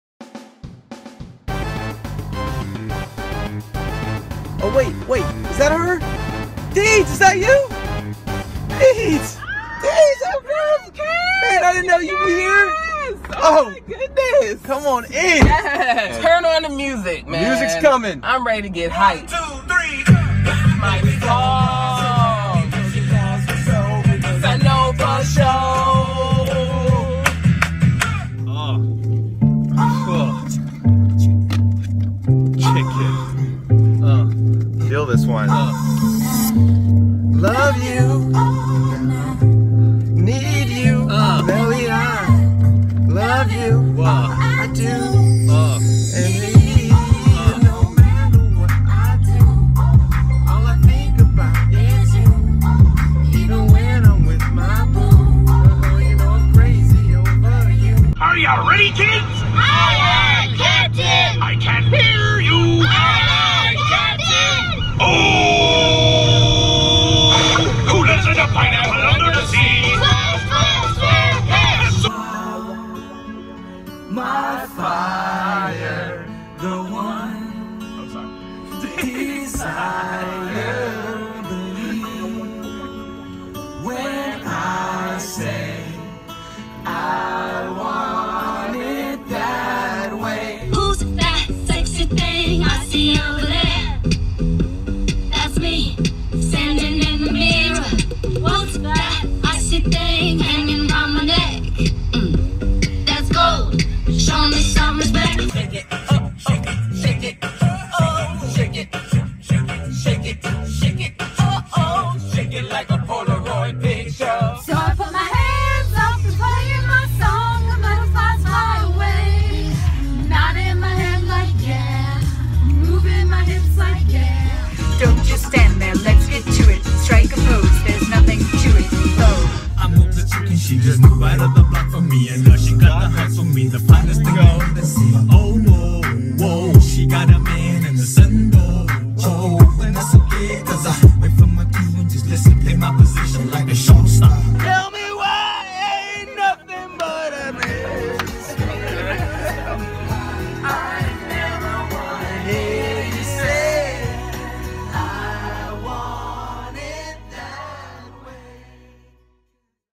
We Know All the Words | Siegfried does Carpool Karaoke!
Sing along if you know the words! At Siegfried we are all about one of our core values which is to have Fun! We are really good at that!